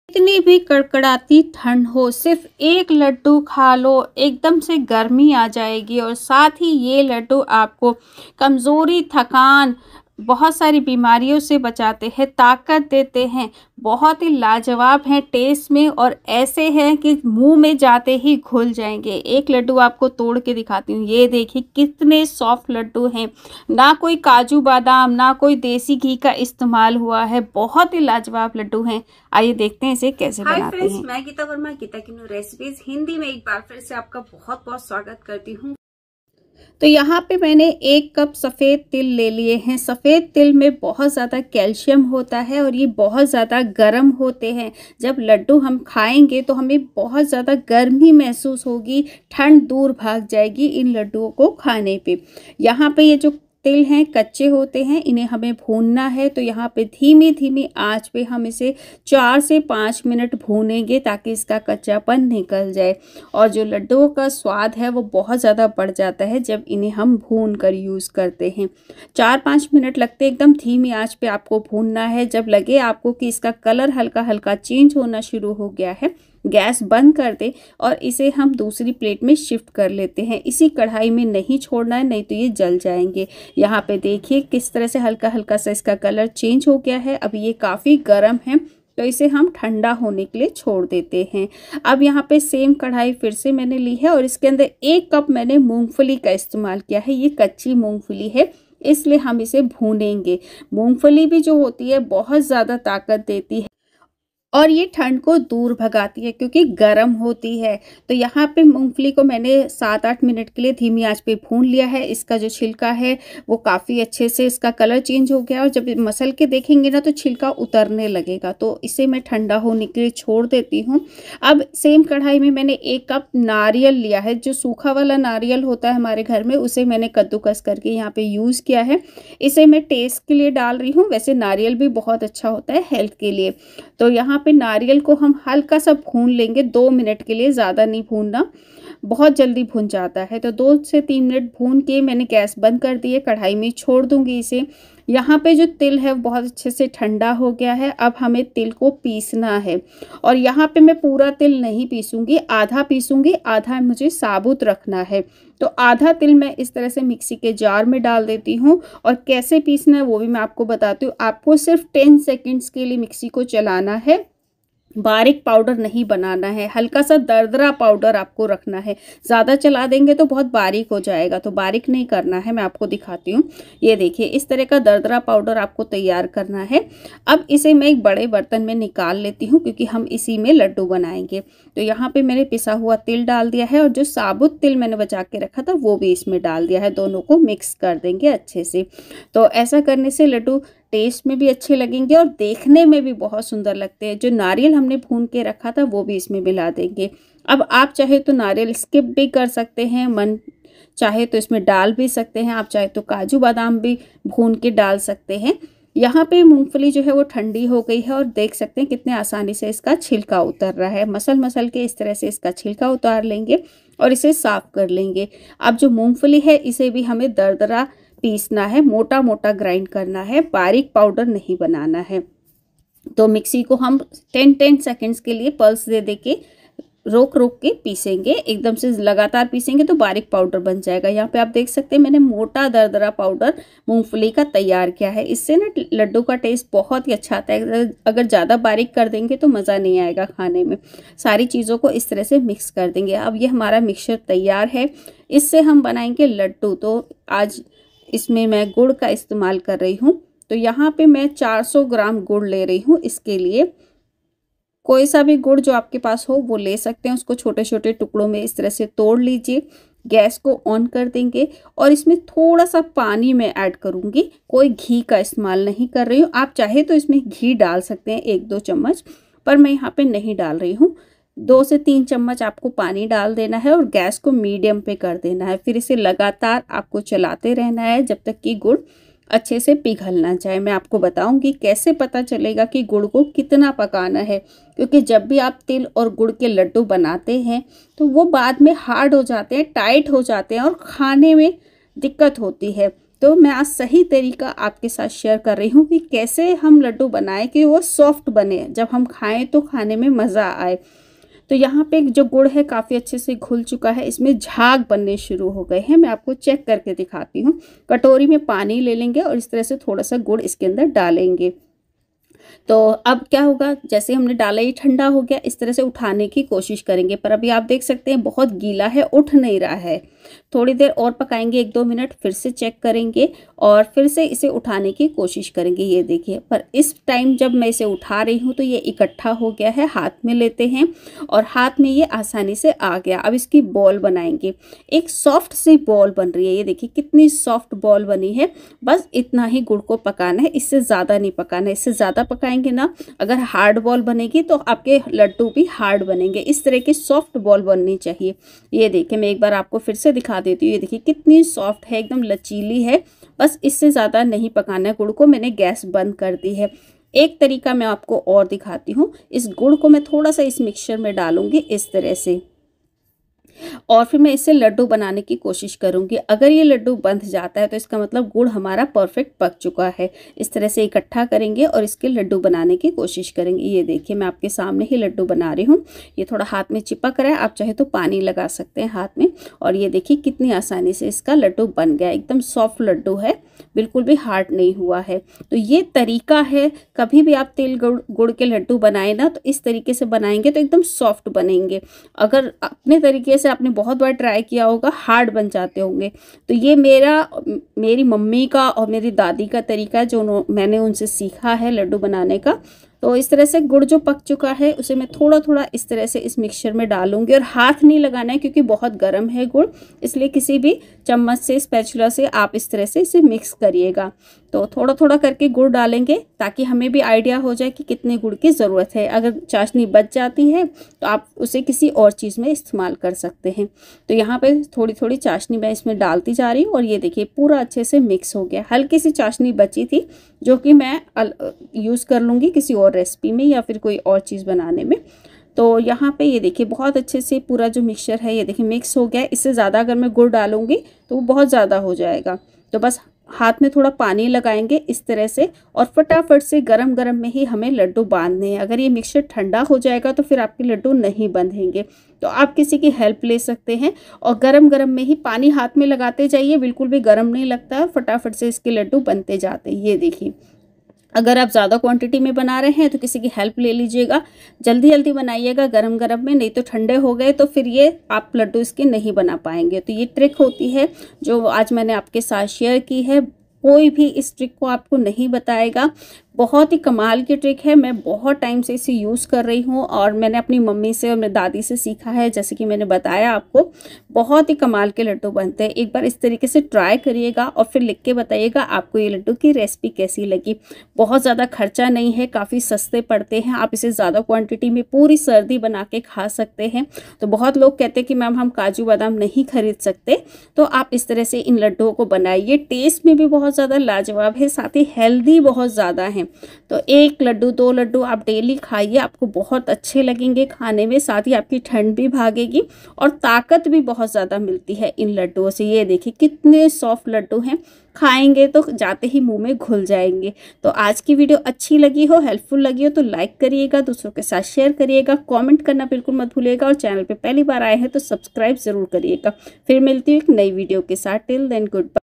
7 दिन ये लड्डू बनाकर खालो कमजोरी थकान कब्ज जोड़ों कमर घुटनों का दर्द दूर करे/Immunity Booster Laddu
सिर्फ 1 लड्डू खालो खासी जुकाम सर्दी कमजोरी थकान खून की कमी दूर करे/Til Gud ke Laddu/Immunity Booster